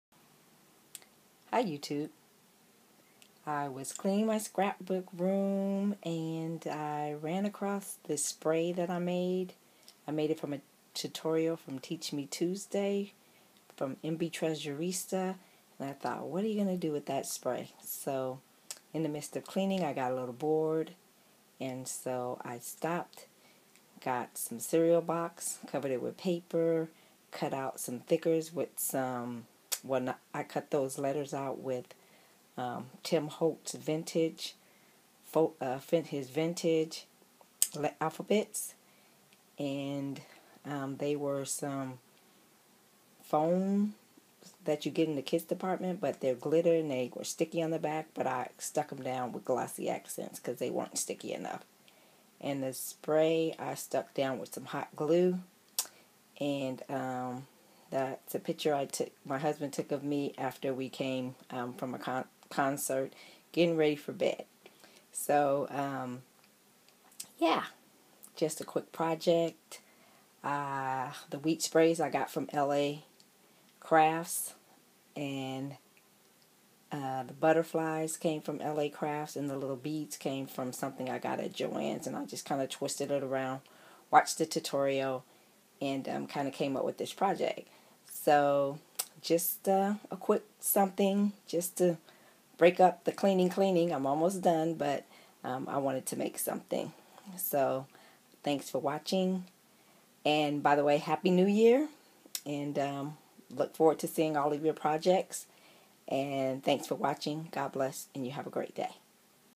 Quick project share Small Layout
I was cleaning my scrapbook room and got bored ran across this spray and stopped and made this project using a cereal box, scraps of paper from my scrap bin, the floral spray I made using a tutorial from teach me tuesdays MB Treasurista. I cut a 7x7 square from the cereal box.  Hope you like it and tfw